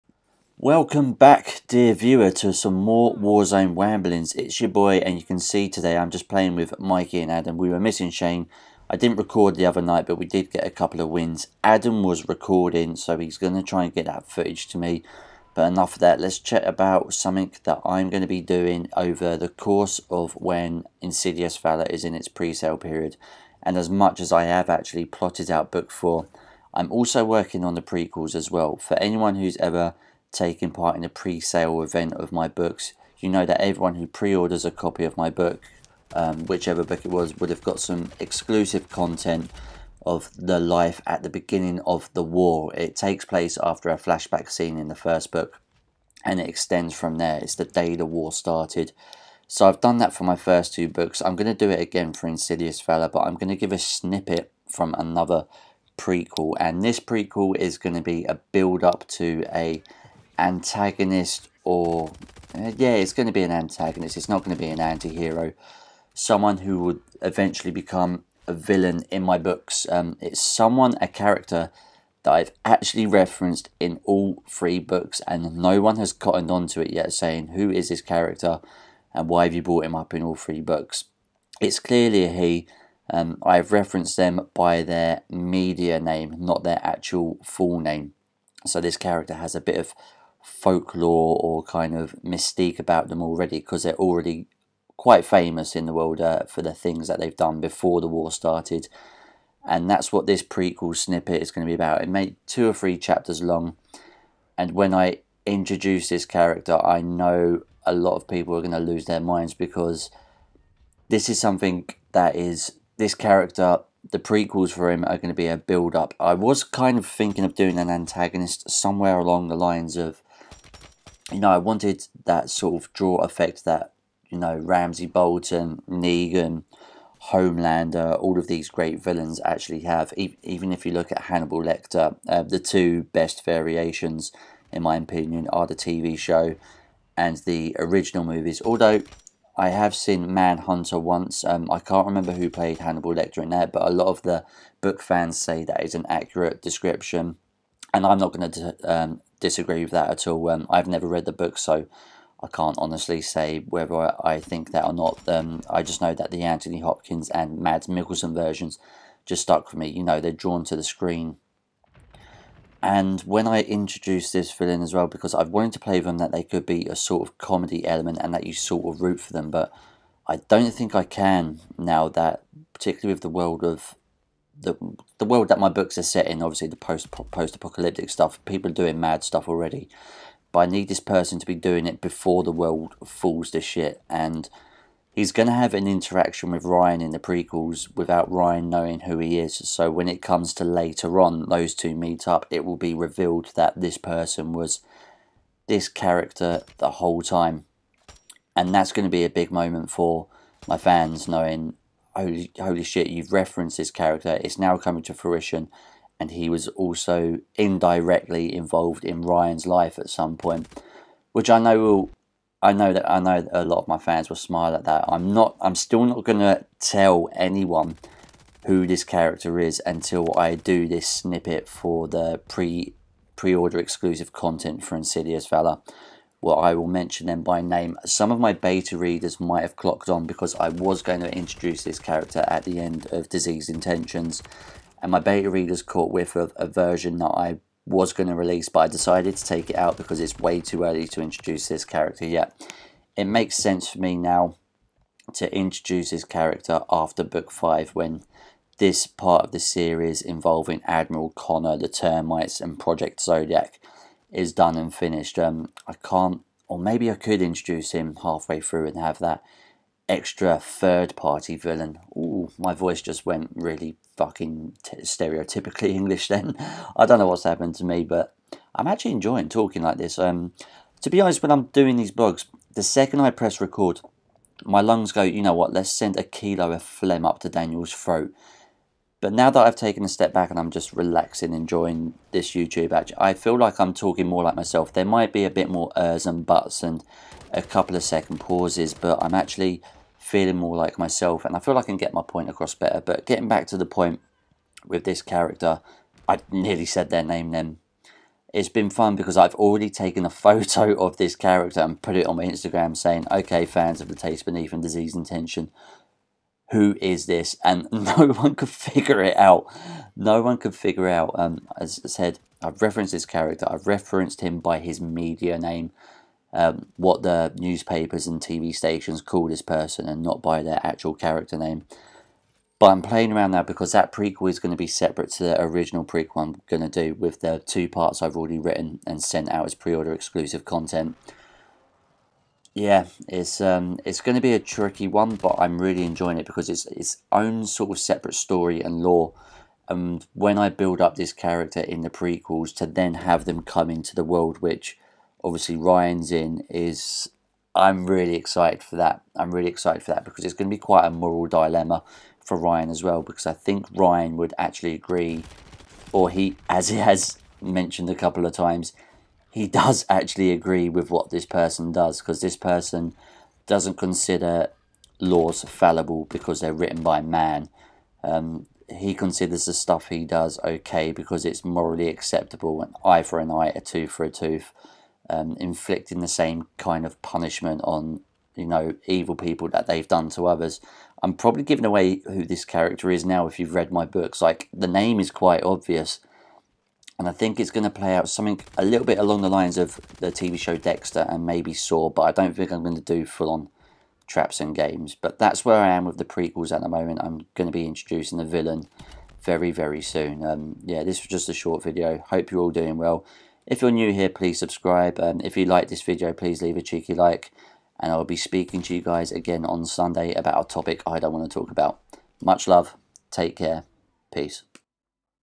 Plotting My Prequels!
Yo team!
After Sunday's video, i thought I'd give you a cheeky little look into what else is going on behind the scenes with my writing. It's at the early stages, and nothing has been fully mapped yet... but it has begun!

The Taste Beneath
Diseased Intentions